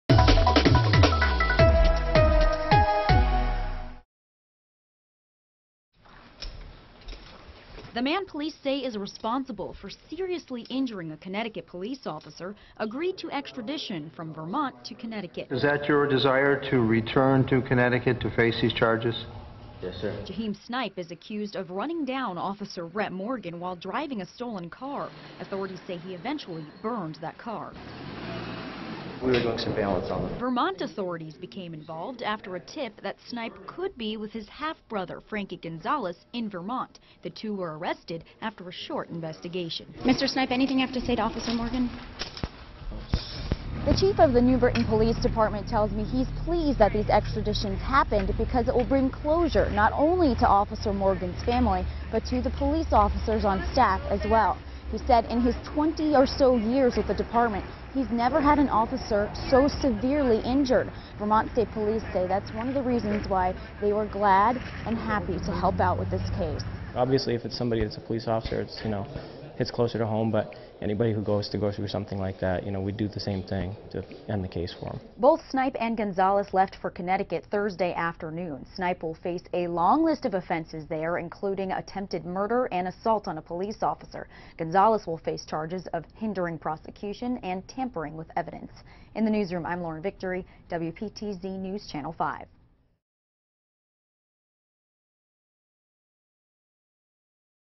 Two accused in Conn. police hit-and-run leaving state
Two men face a Vermont judge for extradition hearings. Police say they are involved in an incident that severely injured a Connecticut police officer.